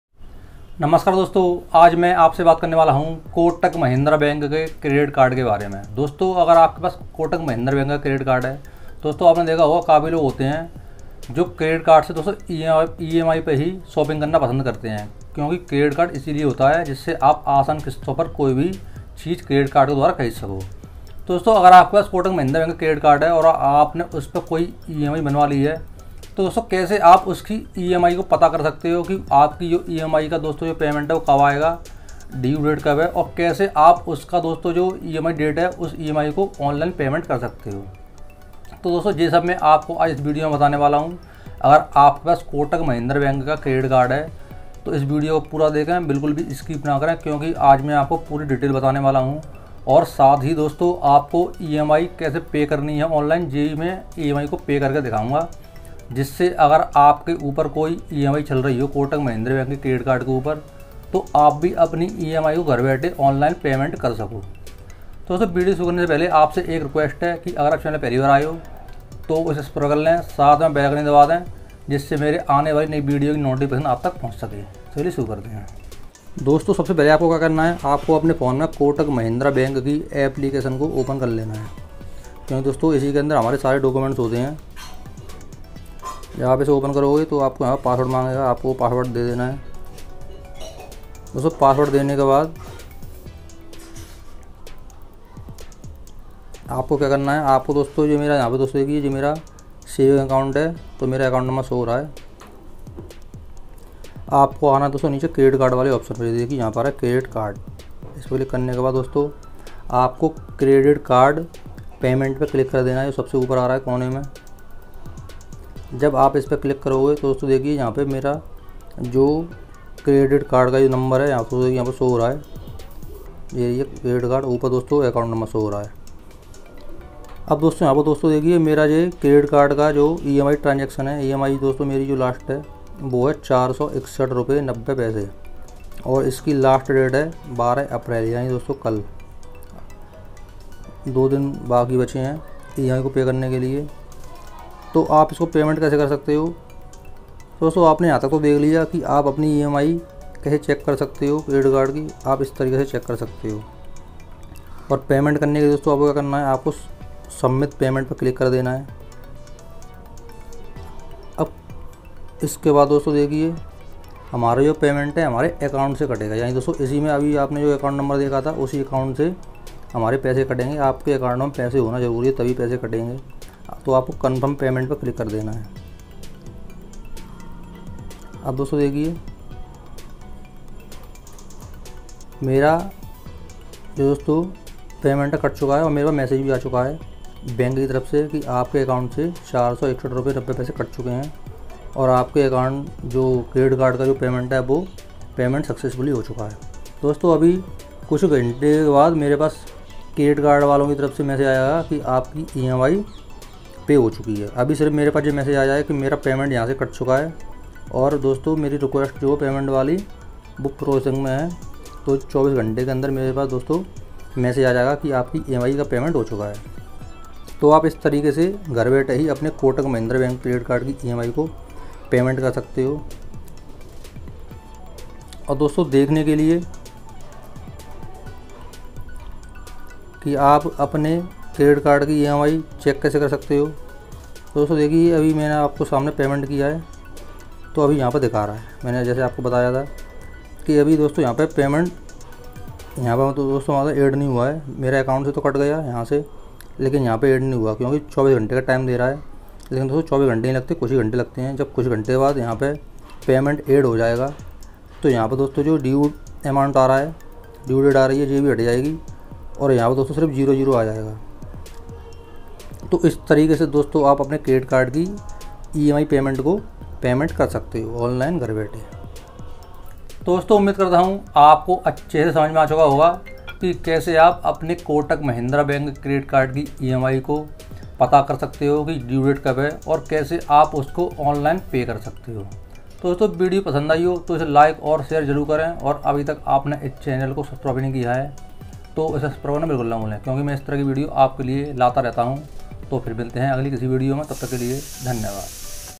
Kotak Mahindra bank credit card emi kaise pata kare, kitni h or last date kab h 🔥🔥
Dosto es video me maine  kotak credit kard ki emi pta krne or online pay krne ka tarika bataya h